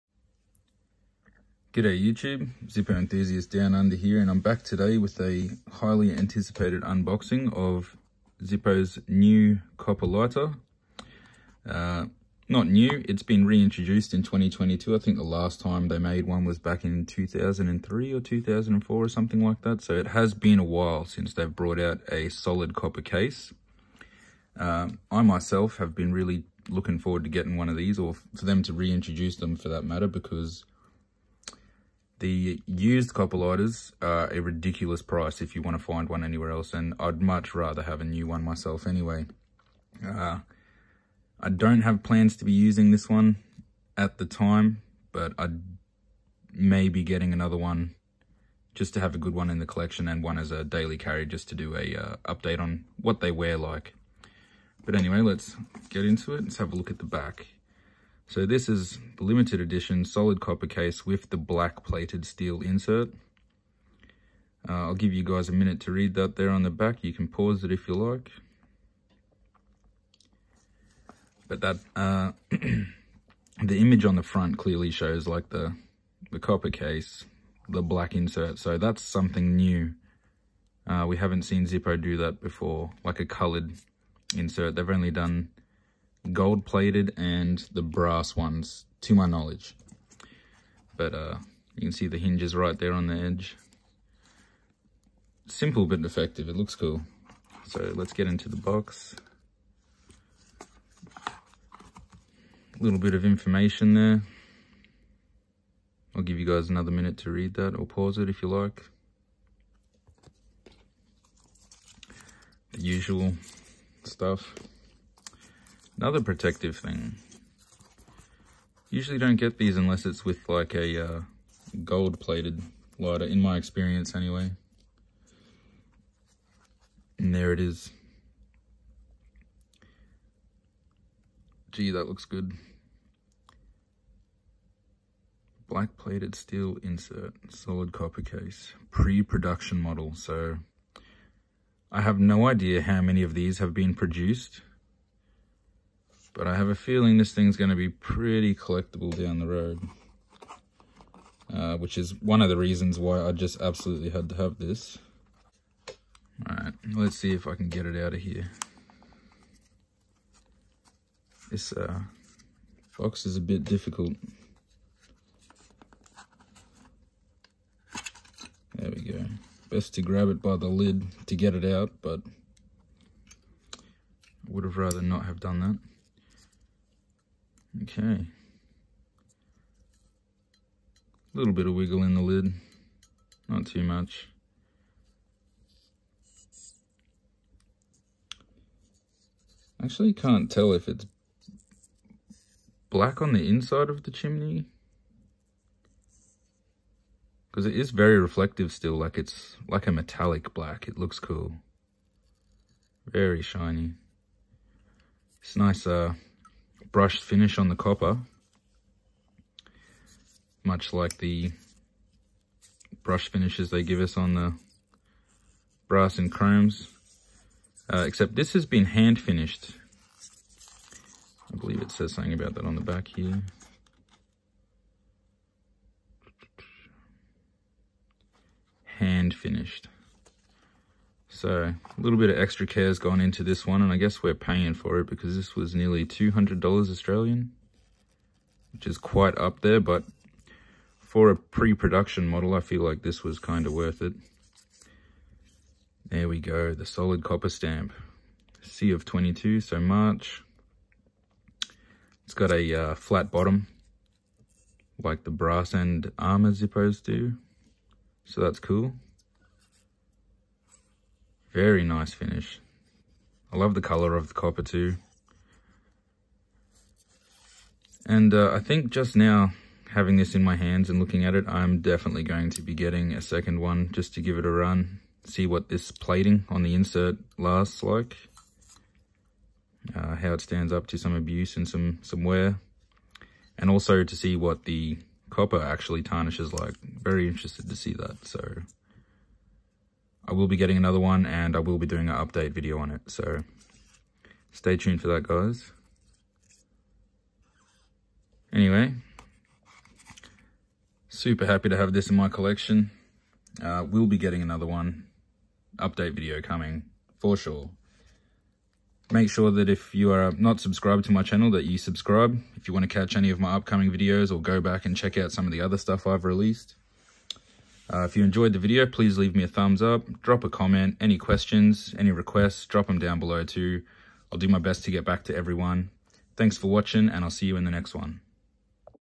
Unboxing New Zippo Solid Copper Pre-Production Model
Breif unboxing video of the Pre-Production Solid Copper Zippo lighter